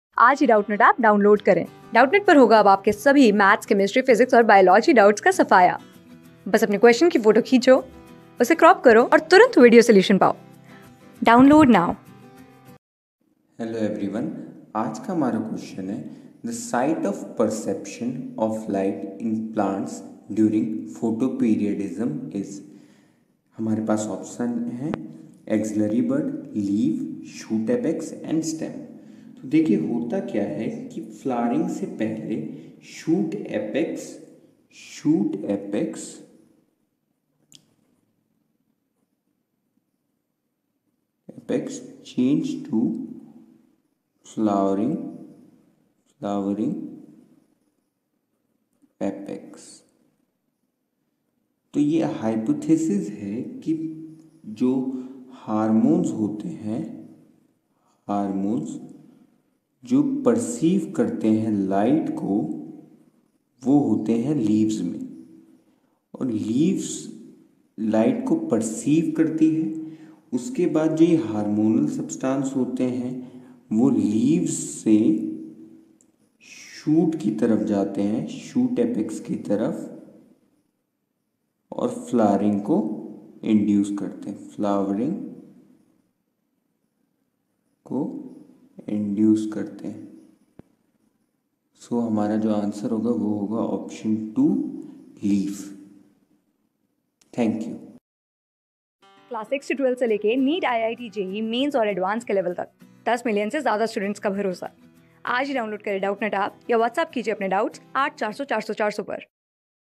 The sight of perception of light in plants during photoperiodism is: | CLASS 12 | NEET 2021 | BI...
The sight of perception of light in plants during photoperiodism is:
Class: 12
Subject: BIOLOGY
Chapter: NEET 2021
Board:NEET

👉 Have Any Query? Ask Us.